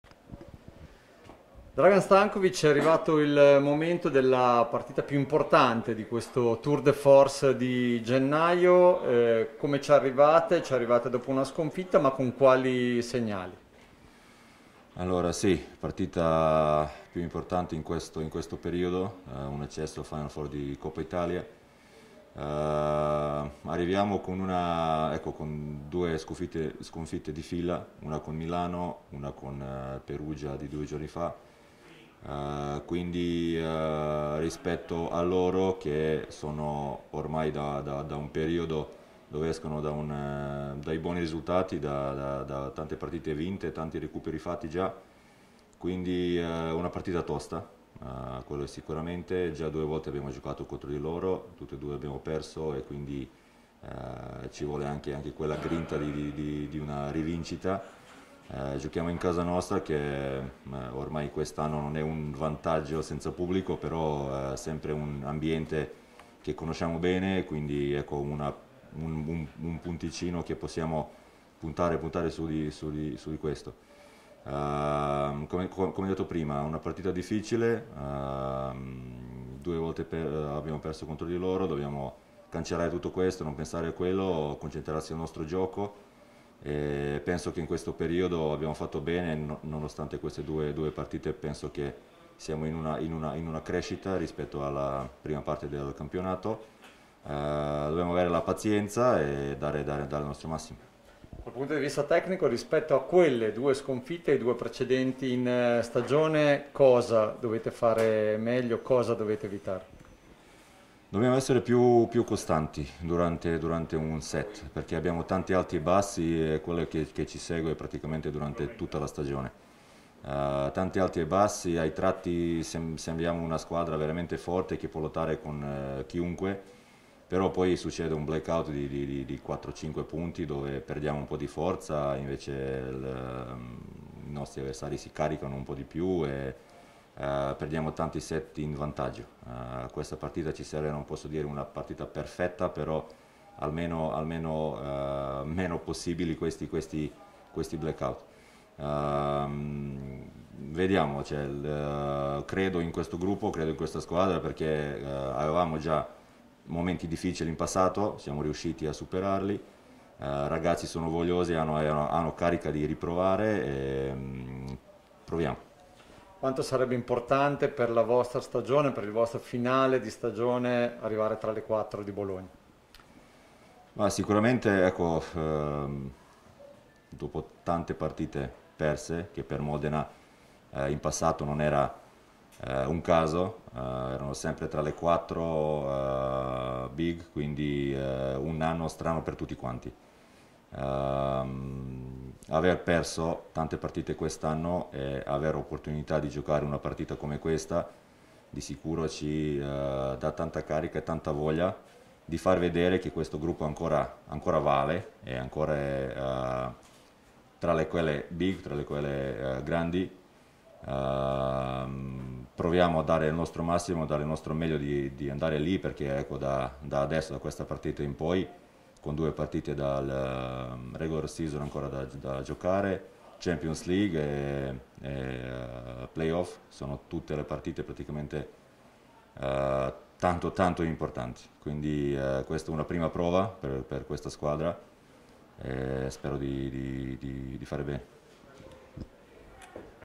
25 gennaio 2021
Stankovic sul match con Monza: "Possiamo e dobbiamo andare in Final Four"